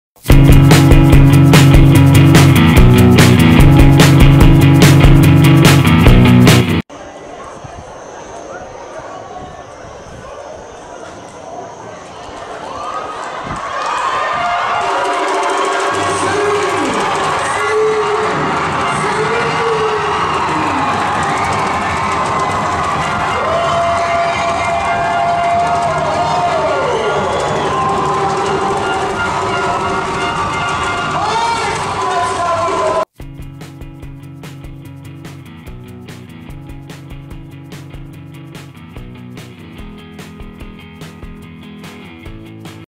Touchdown KVRA da Semana vol. 22 - Eduardo Falcão, Tropa Campina
O jogador do Tropa Campina foi o mais votado entre os nossos fãs do Facebook e foi o vencedor do volume 22 de nossas seleções de touchdown, com uma importante pontuação na primeira vitória do time paraibano na BFA, contra o Recife Pirates.

Gostou do vídeo? Então assine o canal para mais conteúdos como este e fique ligado em nossas mídias para saber tudo sobre o futebol americano nacional: